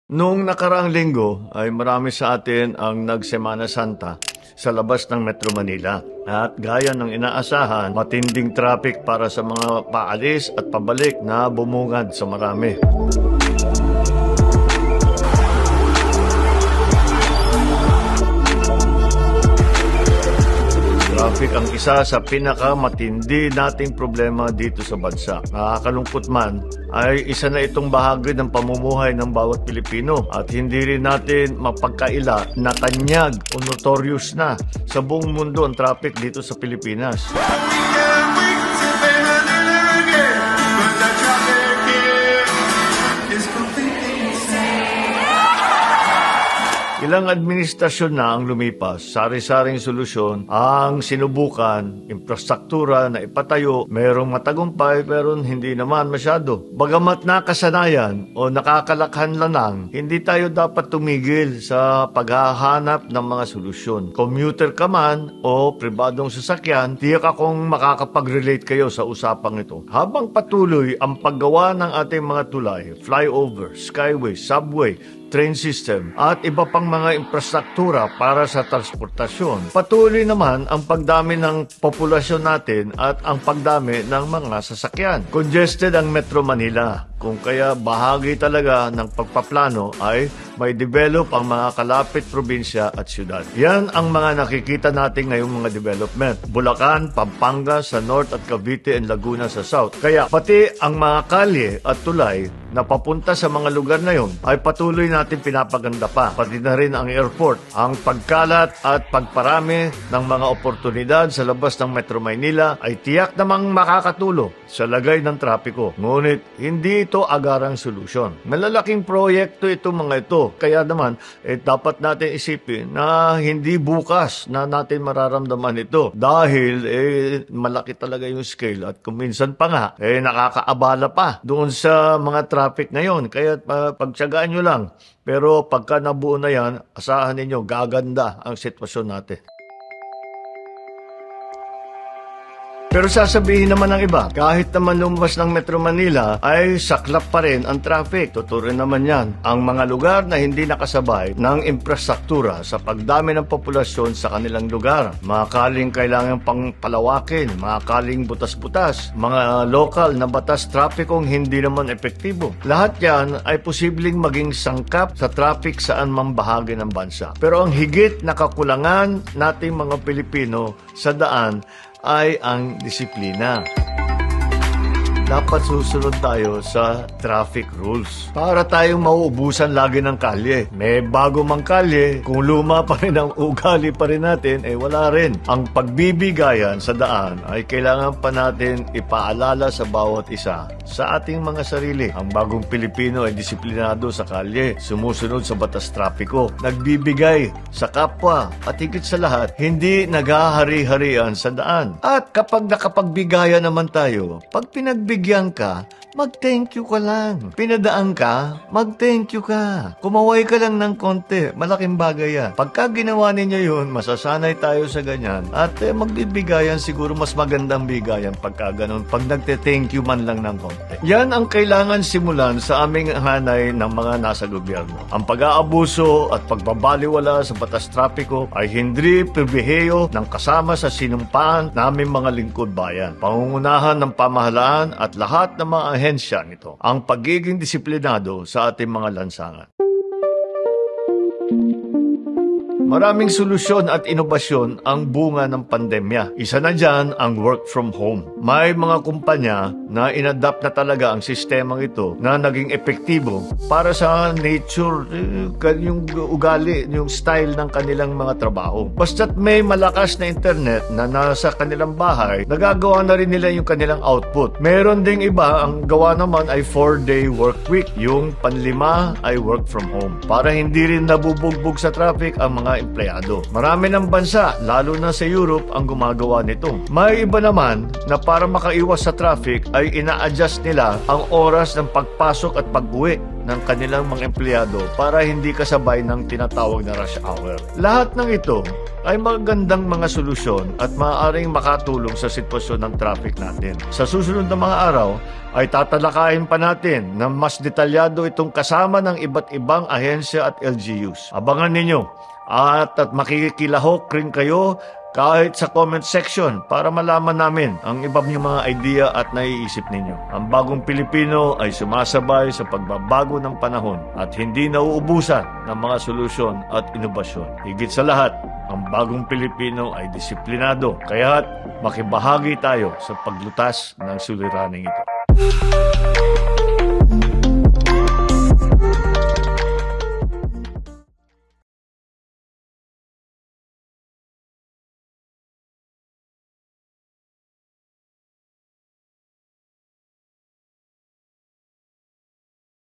WATCH: Marcos addresses traffic woes in PH in latest vlog | ANC
On The Scene: President Ferdinand Marcos Jr. said that government officials are expected to lead by example when it comes to being disciplined drivers, underscoring the importance of abiding by traffic rules to help ease the congestion especially in the capital region.